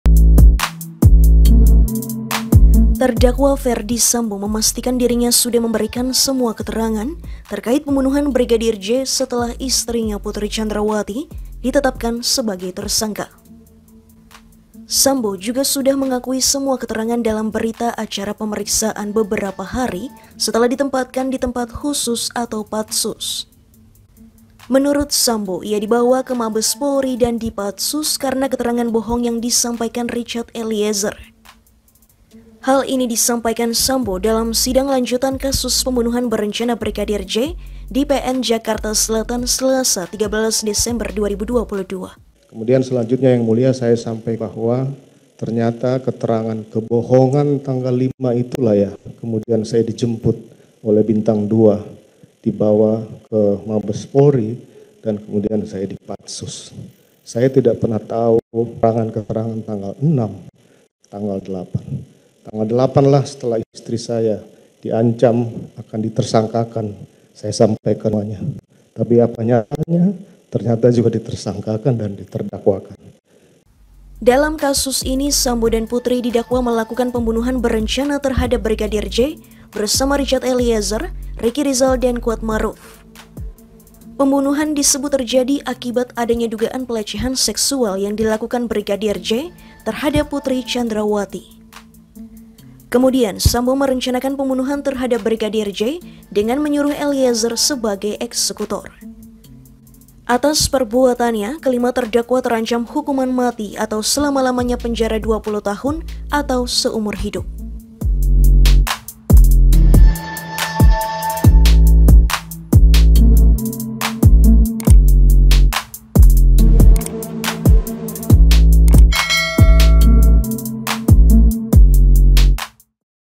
Sambo Baru Cerita Usai Putri Diancam Jadi Tersangka
KOMPAS.com - Ferdy Sambo memastikan dirinya sudah memberikan semua keterangan terkait pembunuhan Brigadir J setelah istrinya, Putri Candrawathi ditetapkan sebagai tersangka.
Hal ini disampaikan Sambo dalam sidang lanjutan kasus pembunuhan berencana Brigadir J di PN Jakarta Selatan, Selasa (13/12/2022).
"Tanggal 8 lah setelah istri saya diancam akan ditersangkakan, saya sampaikan semuanya, tapi apa nyatanya ternyata juga ditersangkakan dan terdakwakan," ucap Sambo.
Sambo juga sudah mengakui semua keterangan dalam berita acara pemeriksaan beberapa hari setelah ditempatkan di tempat khusus (patsus).
Penulis: Rahel Narda Chaterine
Penulis Naskah: Michaela Winda Saputra
Narator: Michaela Winda Saputra
Video Editor: Alfyan Oktora Atmajaya
Produser: Adisty Safitri